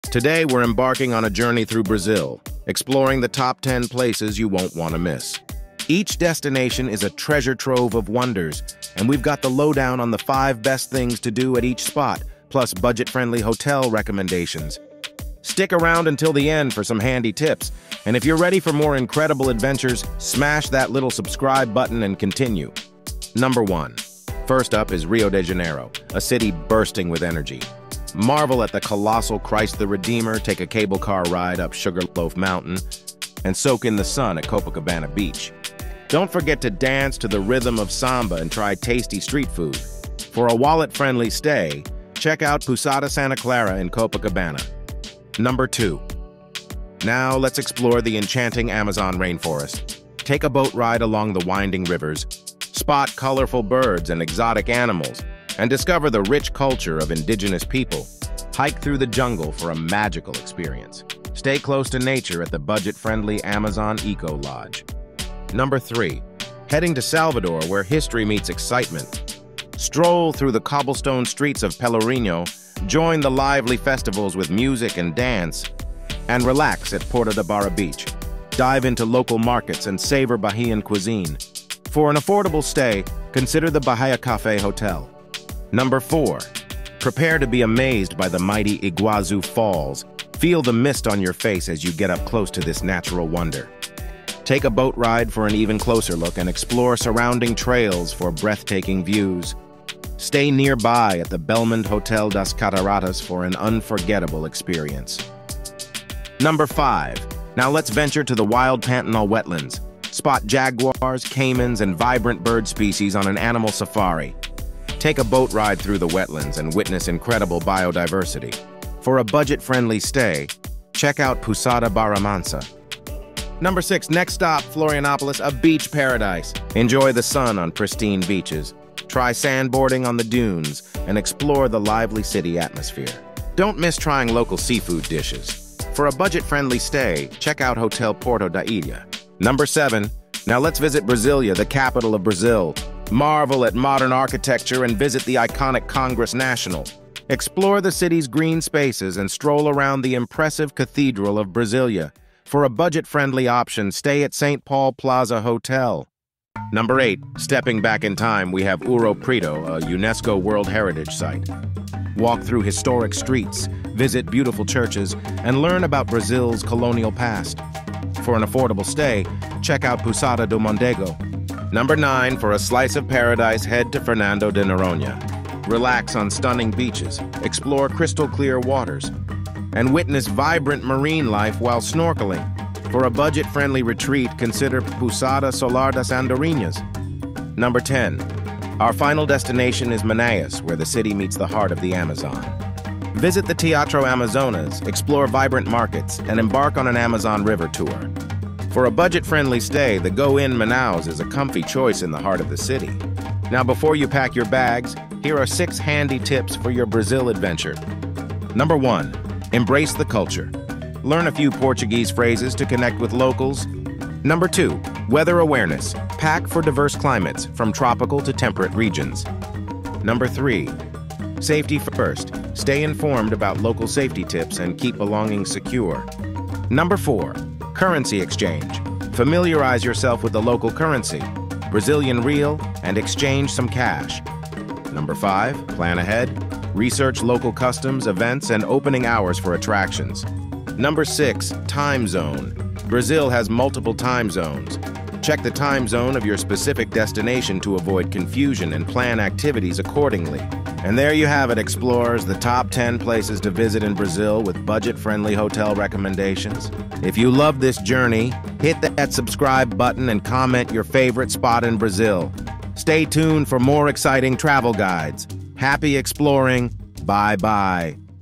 Explore Top 10 Best Places in Brazil | Earth Covery
Top 10 Best Places Brazil. Experience the beauty and culture of Brazil with these top 10 tourist attractions to visit.